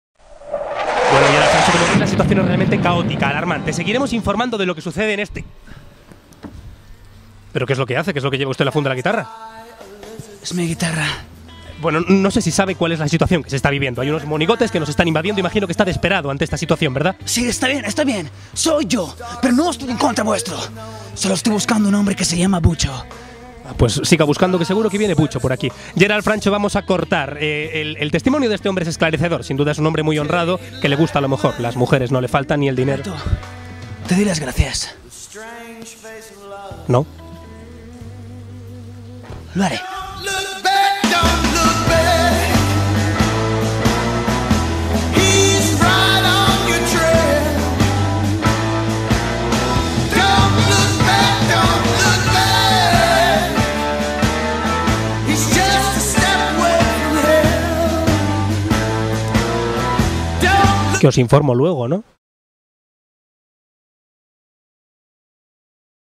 DESPERADO - The Sinflow
Sketch de The Sinflow. Parodia de la pelicula. Realizado por Gerald B. Fillmore & Francho Aijón.